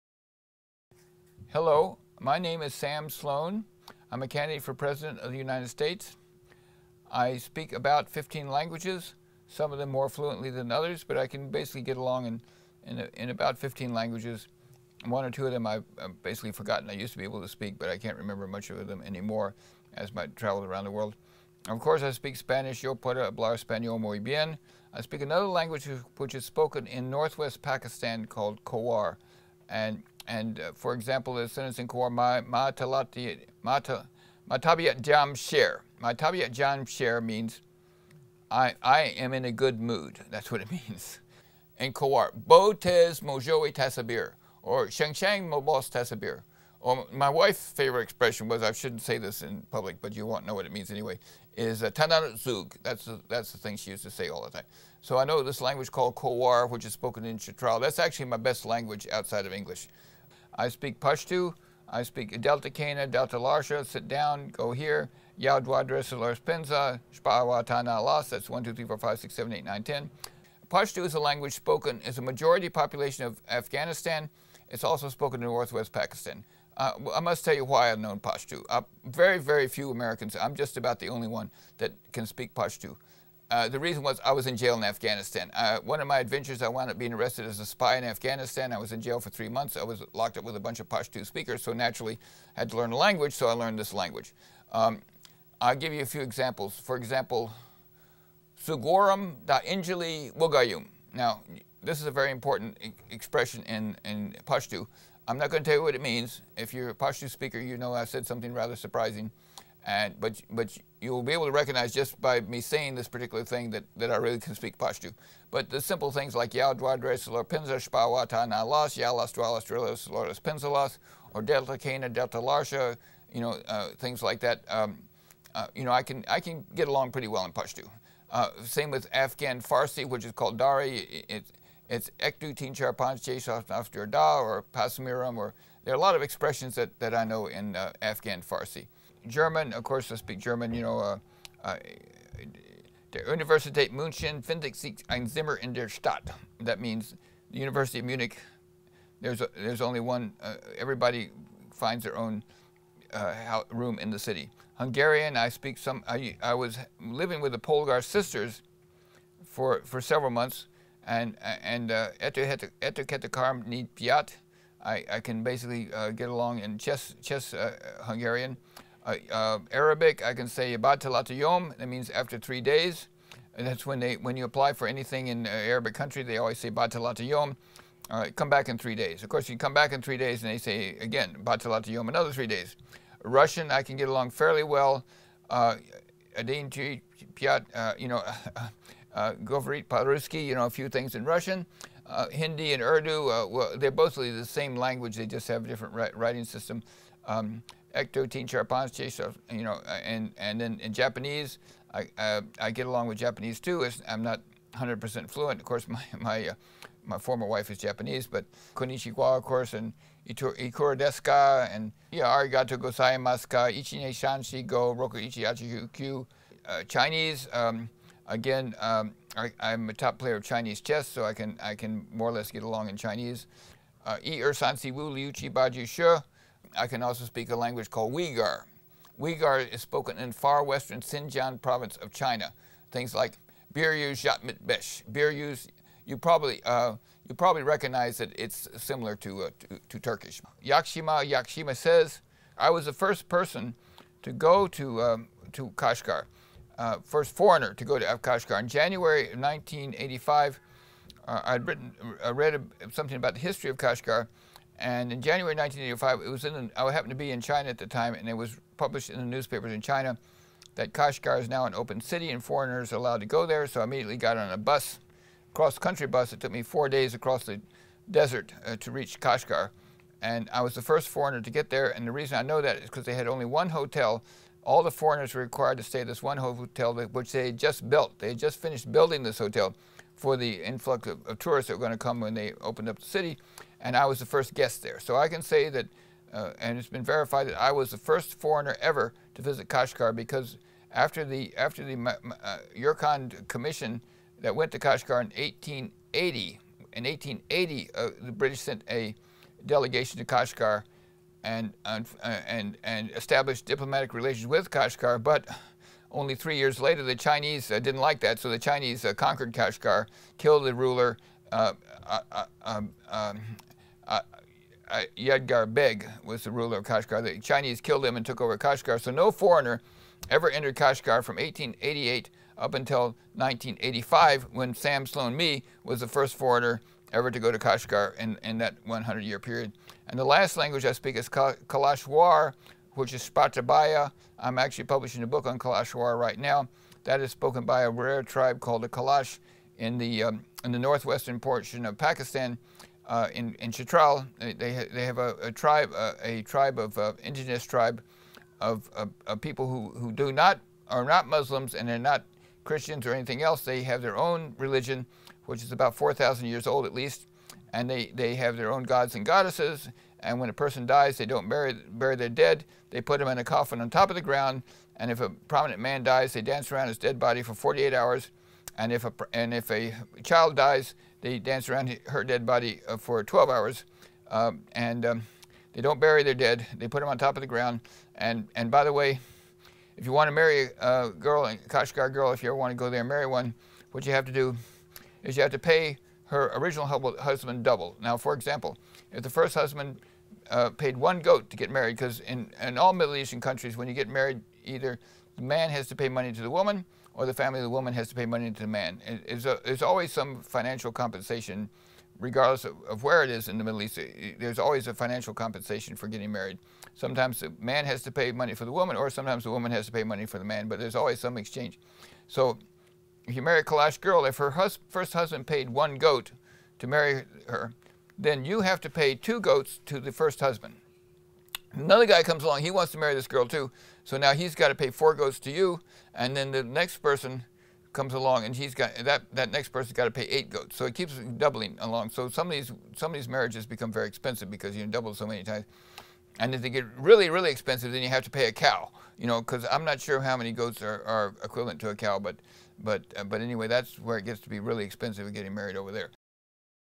Speaking 14 Foreign Languages
I speak the following 14 languages, more or less fluently:
Spanish, Khowar, Pashto, Farsi, German, Hungarian, Arabic, Russian, Hindi, Urdu, Japanese, Chinese, Uyghur, Kalasha.